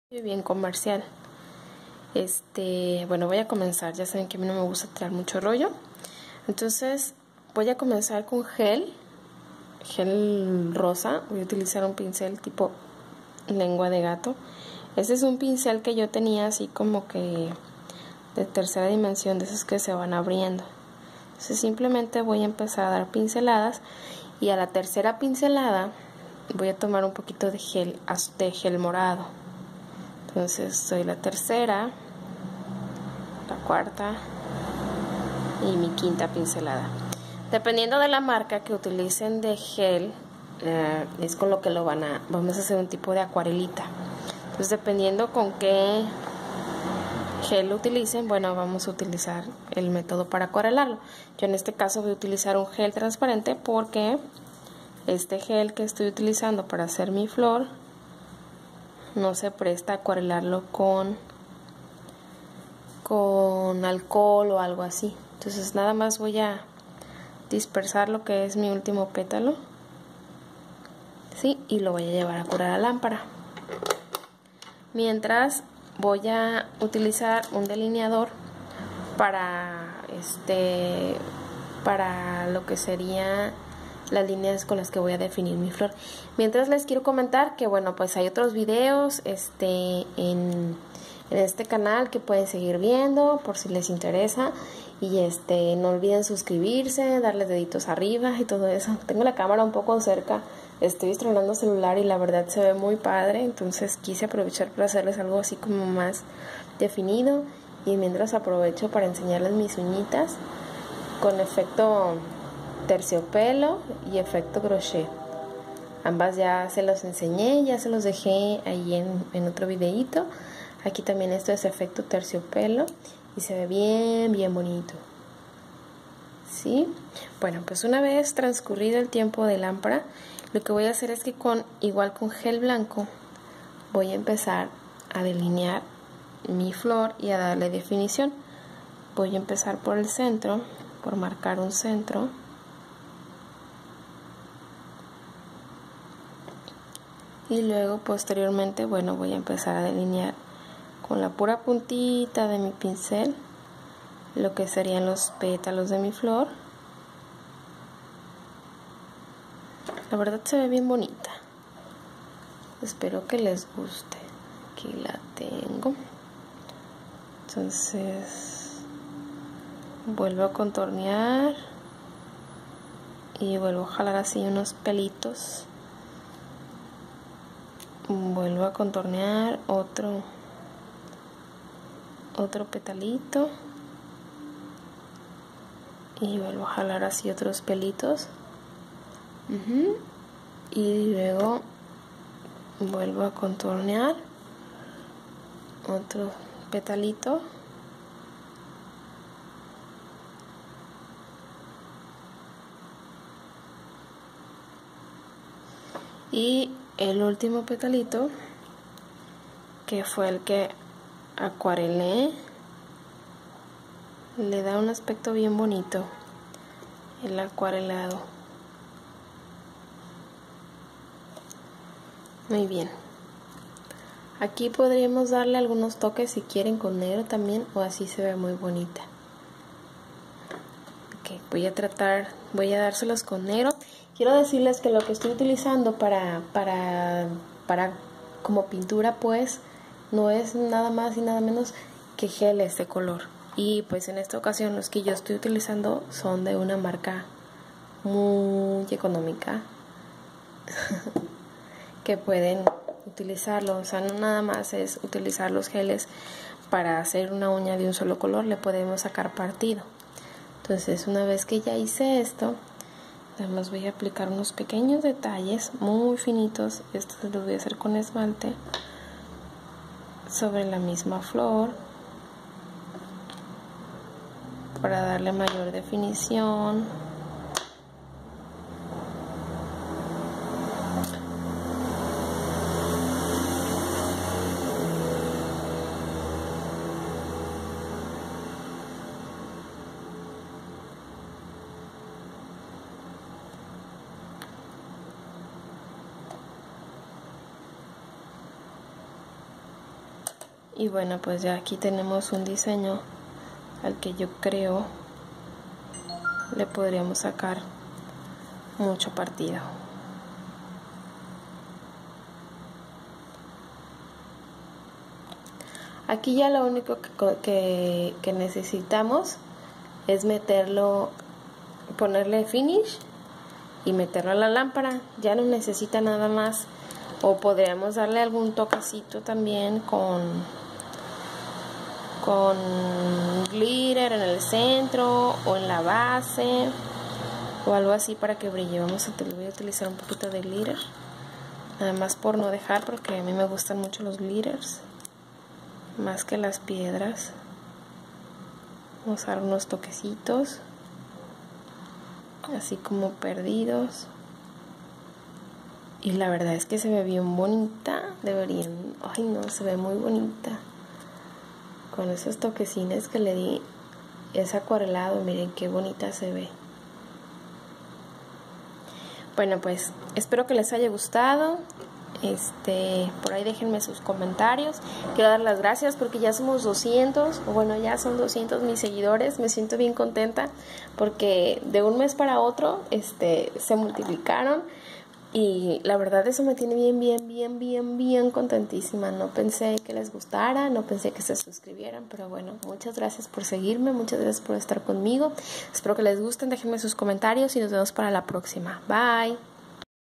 Flor en acuarela con gel polish
Una decoración para sacarle partido a nuestros geles polish